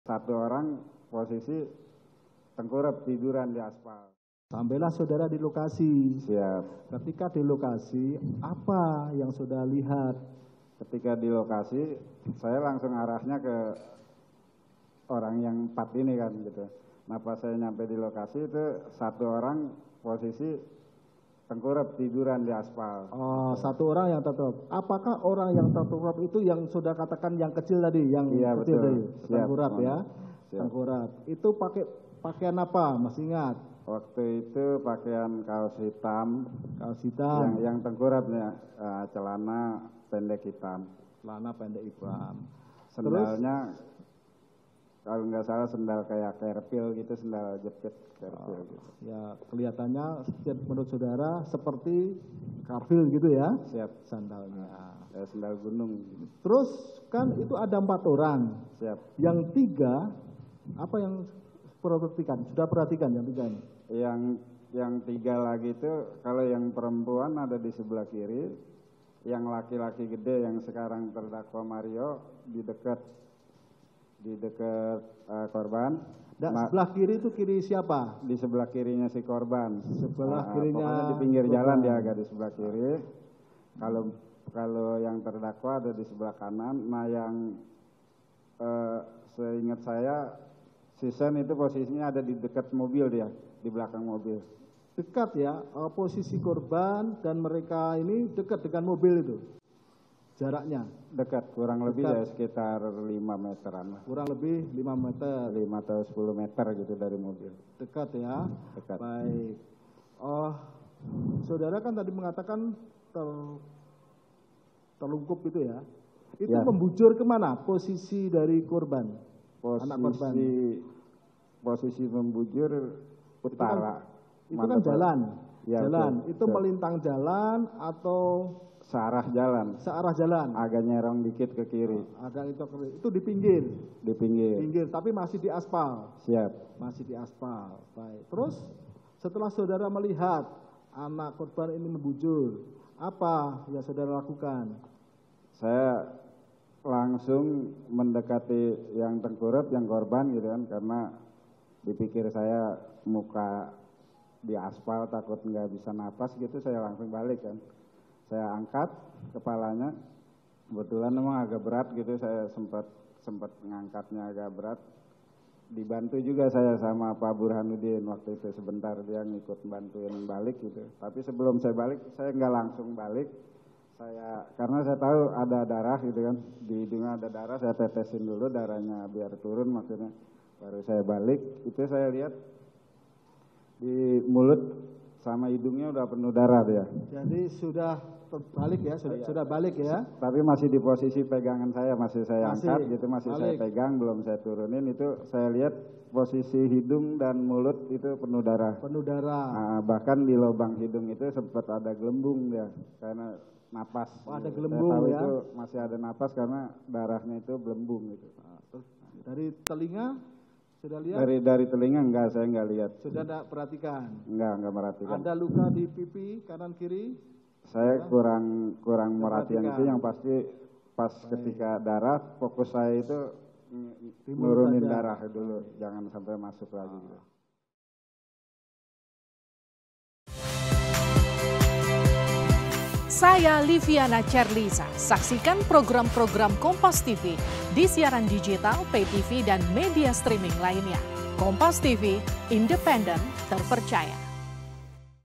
Kesaksian Satpam Kompleks saat Lihat Kondisi David Pertama Kali dalam Sidang Mario Dandy Cs
KOMPAS.TV- Kesaksian Satpam Kompleks saat Lihat Kondisi David Pertama Kali dalam Sidang Mario Dandy Cs

Salah satu saksi yang dihadirkan adalah Abdul Rosyid, seorang Satpam Kompleks.

Dia memberikan kesaksian tentang saat pertama kali melihat David pada saat kejadian.

"Pas saya sampai di lokasi, 1 orang posisi tengkurap tiduran,"

Rosyid menjelaskan bahwa ketika itu ia melihat David dengan hidungnya mengeluarkan darah.